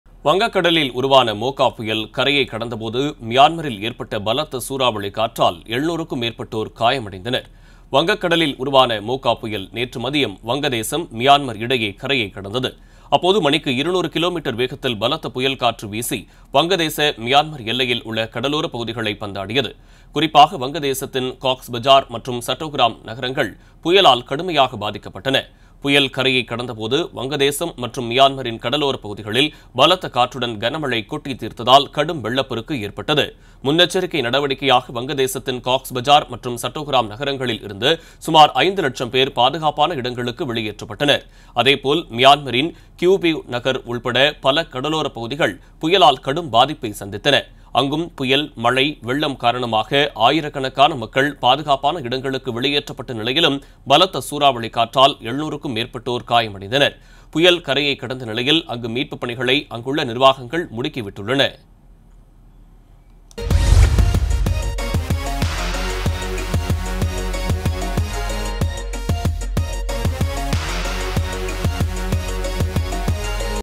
‘மோக்கா’ புயல் பாதிப்பு: மியான்மரில் 700 பேர் காயம் | Mocha Cyclone 700 Injured in Myanmar
‘மோக்கா’ புயல் பாதிப்பு: மியான்மரில் 700 பேர் காயம்